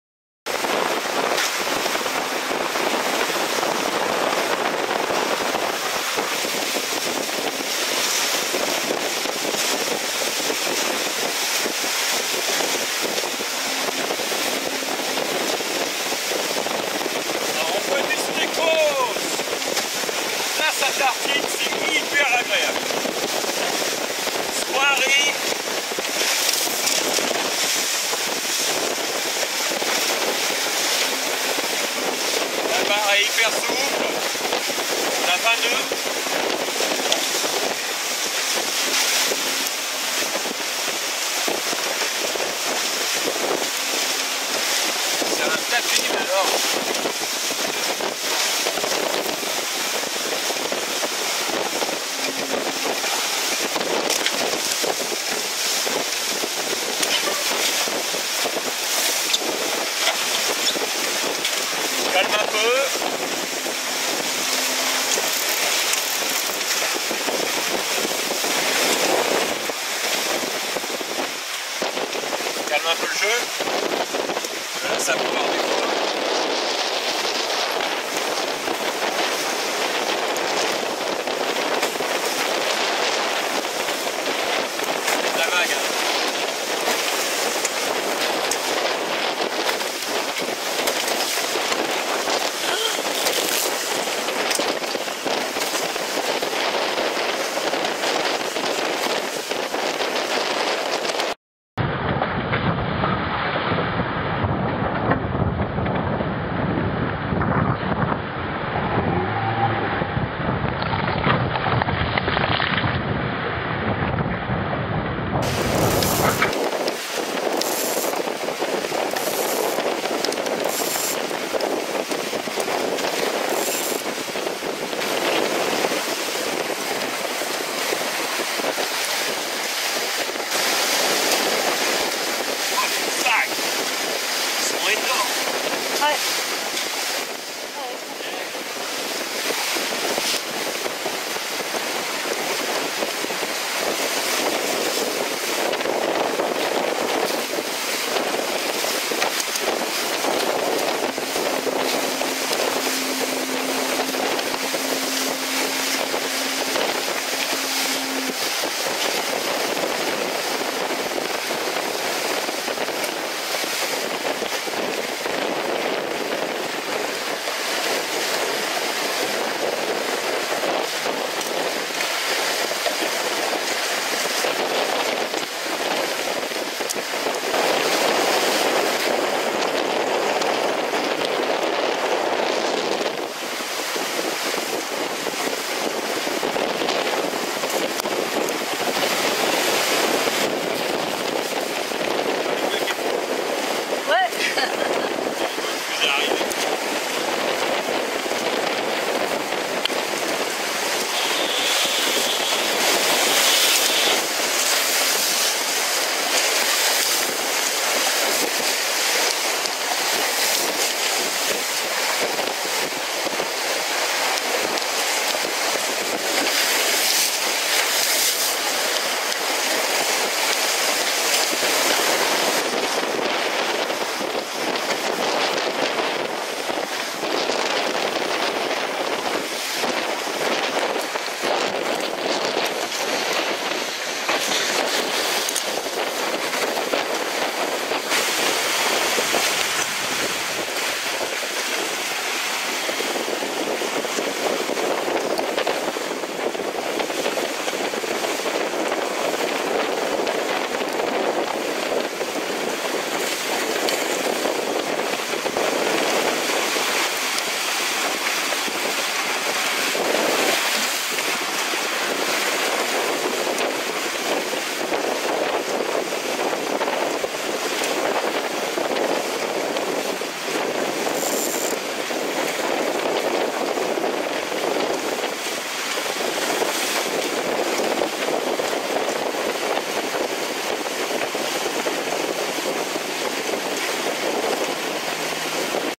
Navigation tonique en TRICAT 25 Evolution
20 noeuds établis, 26 en rafale, des creux de 2 mètres, 1 ris dans la Grand Voile et près de 15 noeuds maxi réguliers...
Et encore, notre Spirit T. ezn avait encore sous les flotteurs mais nous avons joué la sécurité pour un Plaisir total ;-)